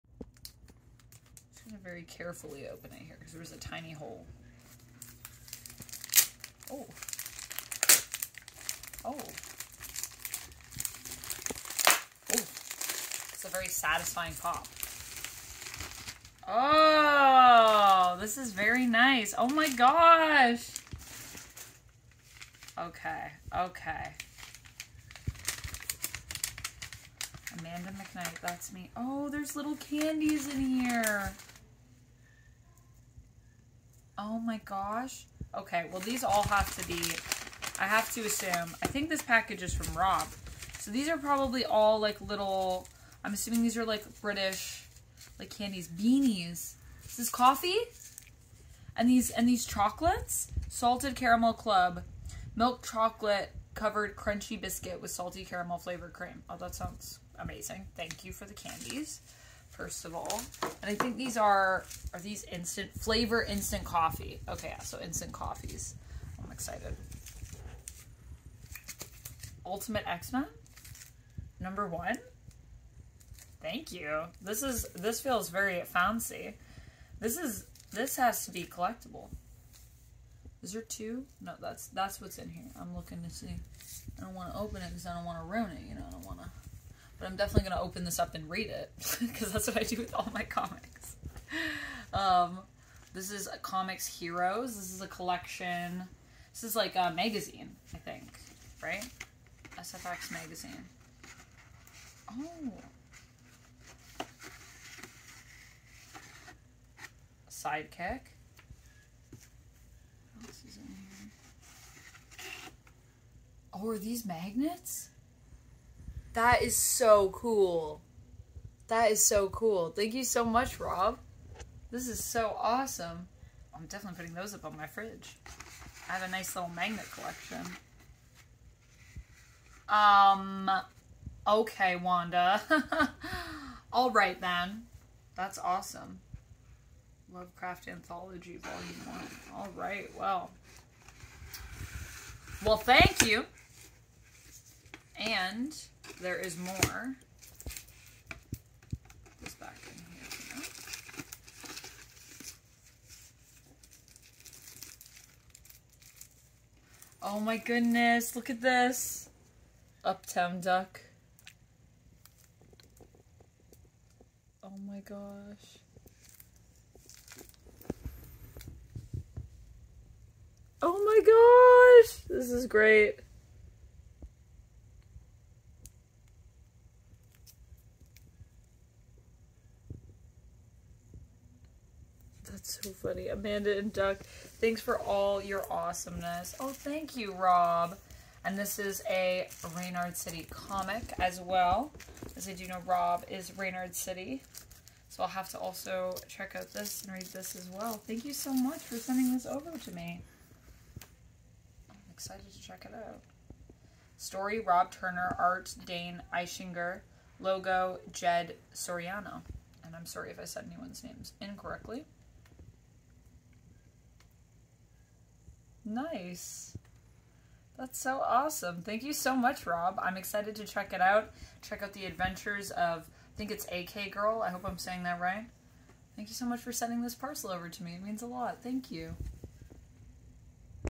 Amanda McKnight issue 7 unboxing
Surprise bonus video- Special guest Amanda McKnight unboxes a special package from us.

Do you want a package like this? You can by buying a print copy of any of our comics or by getting the $10 Fox Box Patreon tier Also special thanks to Canary Comics for the comic items!